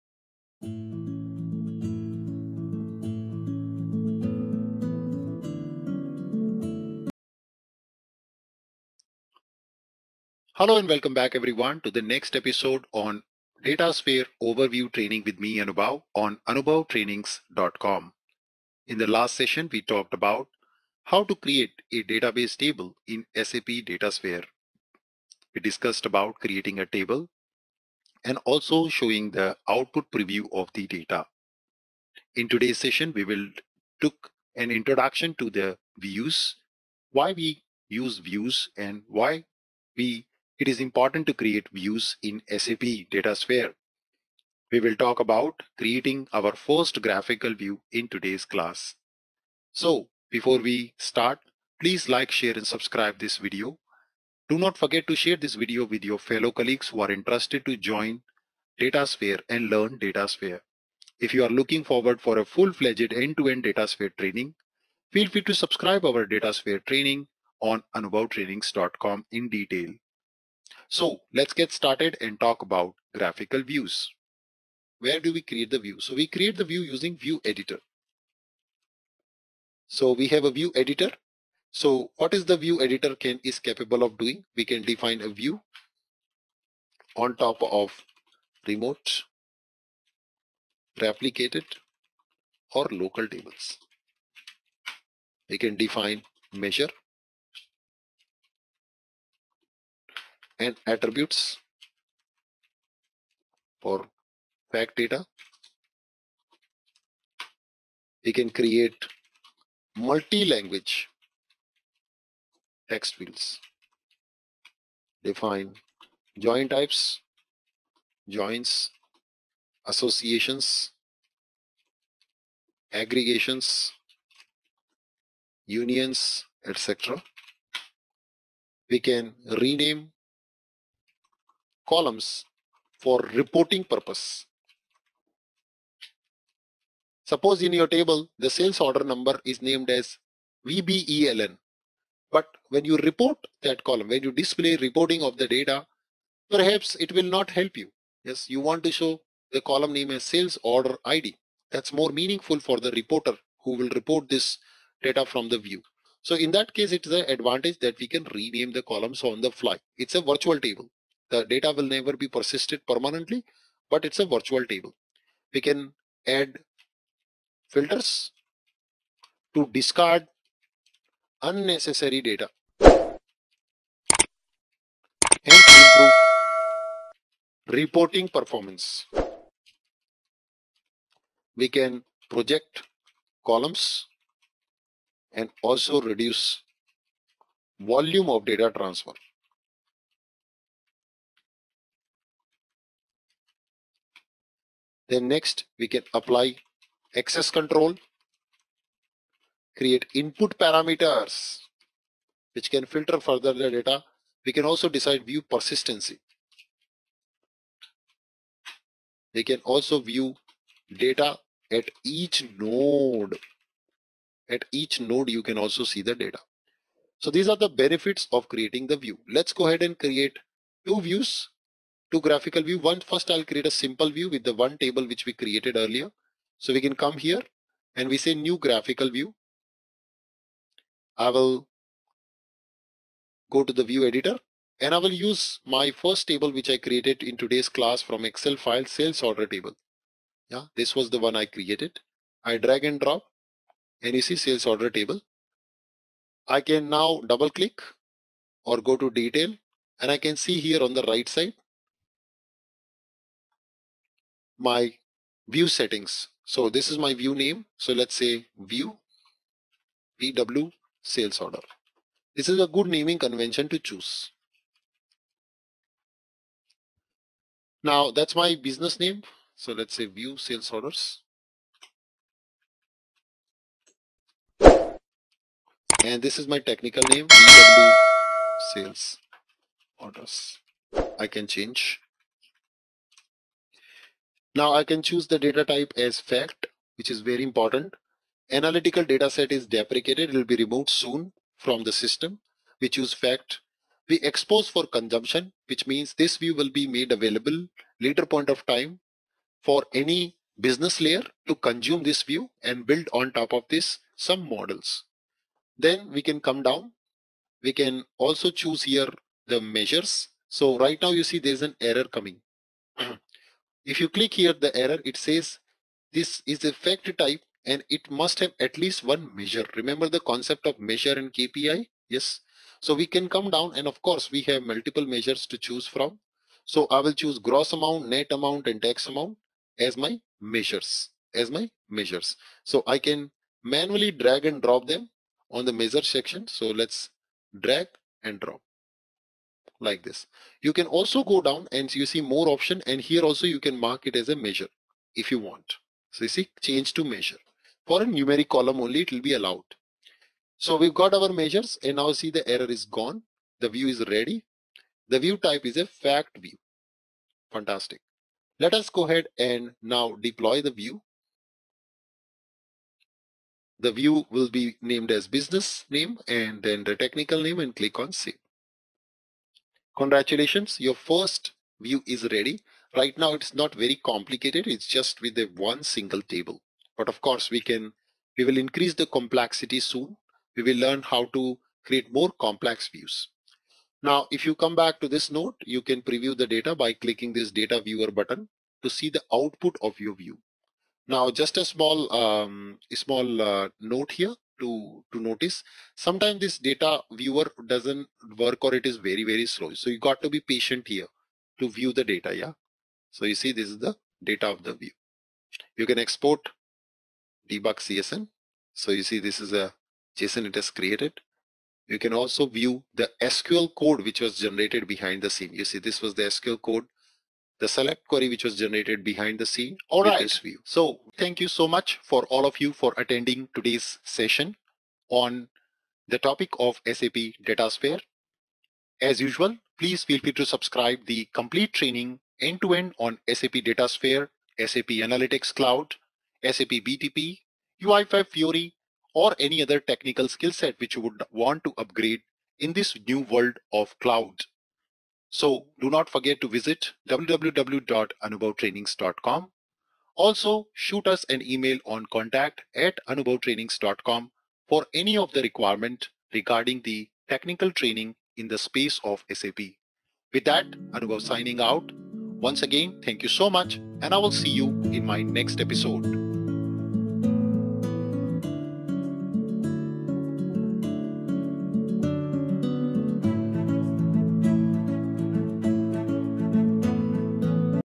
08 Create Graphical views in SAP Datasphere | Preview and Transform Your Data in a Graphical View
In this Video, we will demonstrate how to create a view in SAP Datasphere with a dimension semantic usage. You will learn how to create the dimensional view, modify the semantic type to add context to columns, calculate a geo-coordinate column to display map locations, and configure a level-based hierarchy to group the table members.
SAP DataSphere empowers you to effortlessly create data views through a user-friendly graphical interface. With drag-and-drop functionality from the Source Browser, seamless data joining, transformation capabilities, and customizable output structures, you gain full control of your data processing and visualization.